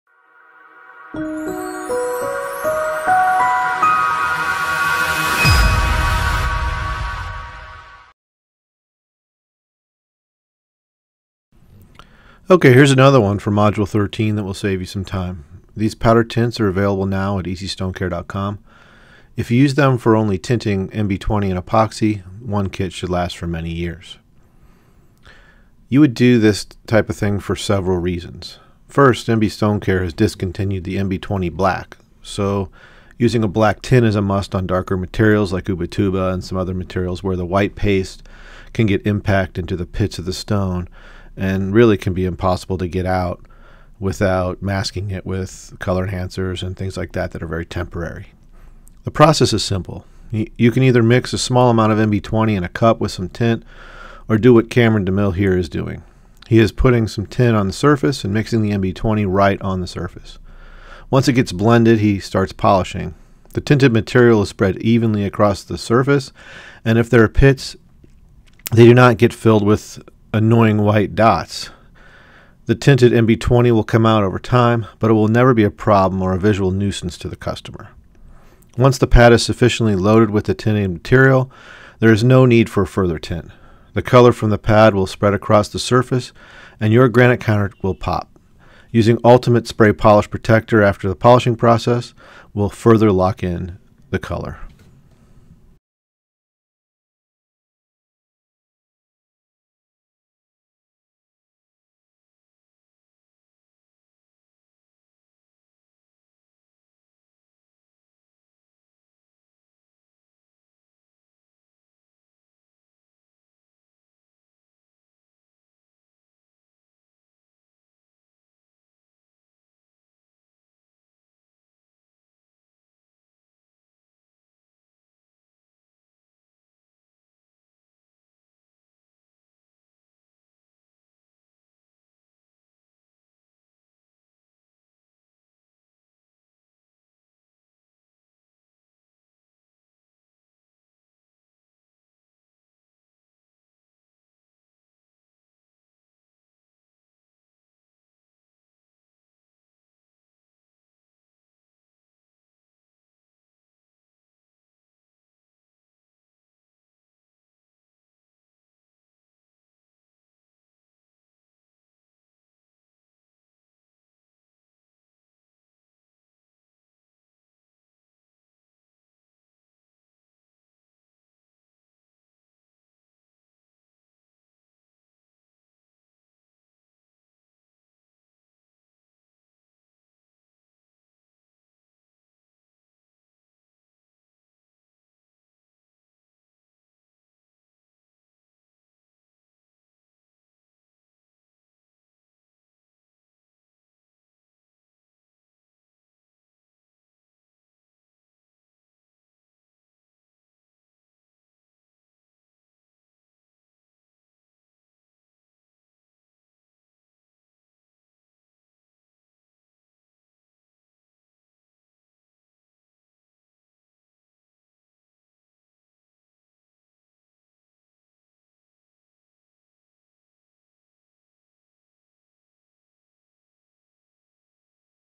Using Powdered Tint with MB20 to Polish Granite
This short one shows Cameron DeMille using Bonstone powdered tints with MB20 to polish granite and make sure that the surface is left without the white spots so prevalent when using this type of polish on darker granites with small pits.